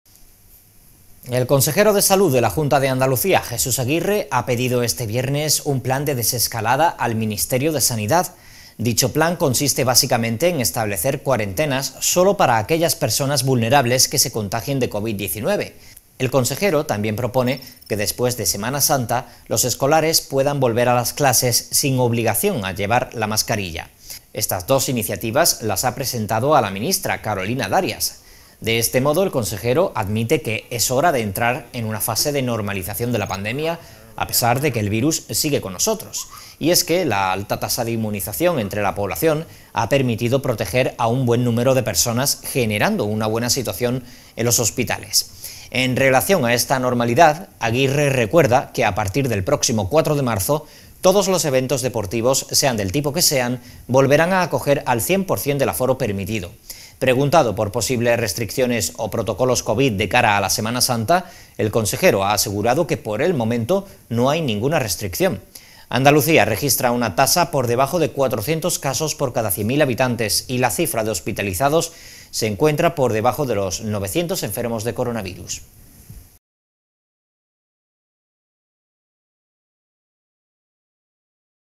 🔴Noticia - Andalucía plantea normalizar la pandemia quitando la mascarilla
El consejero de Salud de la Junta de Andalucía, Jesús Aguirre, ha pedido este viernes un plan de desescalada al Ministerio de Sanidad. Dicho plan, consiste básicamente en establecer cuarentenas sólo para aquellas personas vulnerables que se contagien de Covid-19. El consejero también propone que después de Semana Santa, los escolares puedan volver a las clases sin obligación de llevar la mascarilla. Estas dos iniciativas las ha presentado a la ministra, Carolina Darias. De este modo, el consejero, admite que es hora de entrar en una fase de normalización de la pandemia a pesar de que el virus siga con nosotros. Y es que la alta tasa de inmunización entre la población ha permitido proteger a un buen número de personas generando una buena situación en los hospitales. En relación a esta normalidad, Aguirre recuerda que a partir del próximo 4 de marzo, todos los eventos deportivos, sean del tipo que sea, volverán a acoger al 100% del aforo permitido. Preguntado por posibles restricciones o protocolos covid de cara a la Semana Santa, el consejero ha asegurado que por el momento no hay ninguna restricción. Andalucía registra una tasa por debajo de 400 casos por cada 100.000 habitantes y la cifra de hospitalizados se encuentra por debajo de los 900 enfermos de coronavirus.

English:

The Minister of Health of the Junta de Andalucía, Jesús Aguirre, has requested this Friday a de-escalation plan from the Ministry of Health. Said plan basically consists of establishing quarantines only for those vulnerable people who become infected with Covid-19. The counselor also proposes that after Easter, schoolchildren can return to class without the obligation to wear a mask. These two initiatives have been presented to the minister, Carolina Darias. In this way, the counselor admits that it is time to enter a phase of normalization of the pandemic despite the fact that the virus is still with us. And it is that the high rate of immunization among the population has made it possible to protect a good number of people, generating a good situation in hospitals. In relation to this normality, Aguirre recalls that as of March 4, all sporting events, whatever their type, will once again host 100% of the permitted capacity. Asked about possible restrictions or covid protocols for Easter, the counselor has assured that at the moment there are no restrictions. Andalusia registers a rate below 400 cases per 100,000 inhabitants and the number of hospitalized patients is below 900 coronavirus patients.

Live streaming